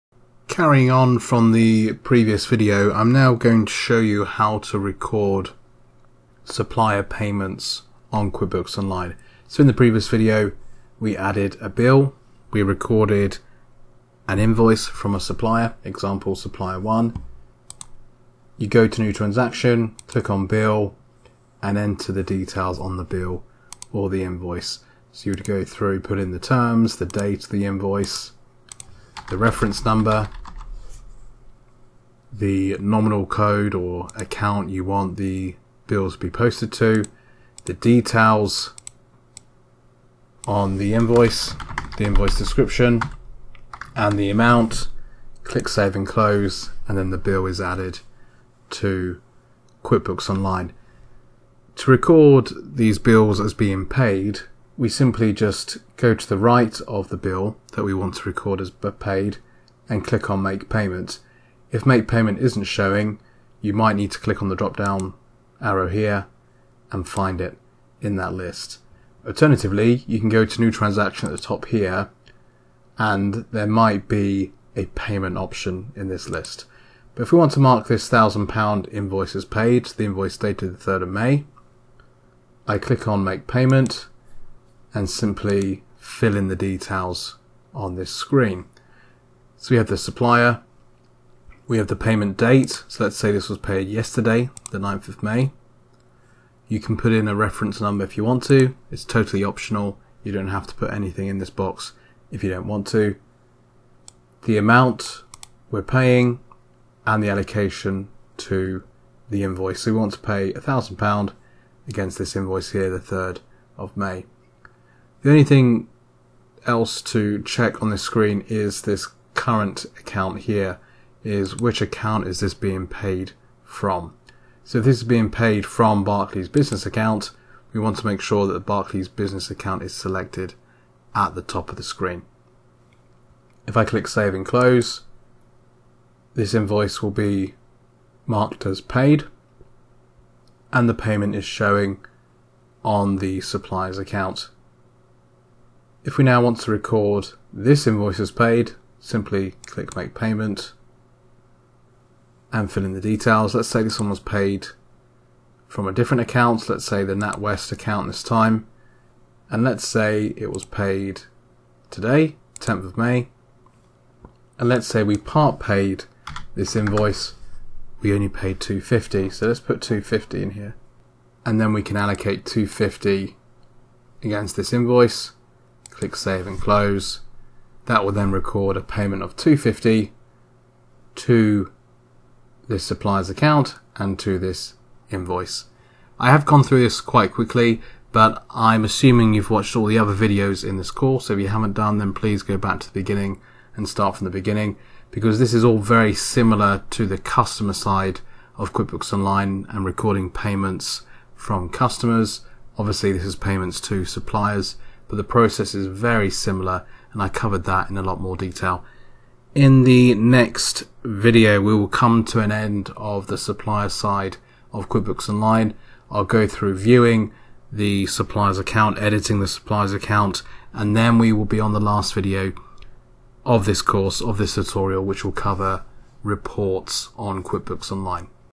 Recording Supplier Payments - QuickBooks Online Tutorial - Part 19
How to record supplier payments on QuickBooks Online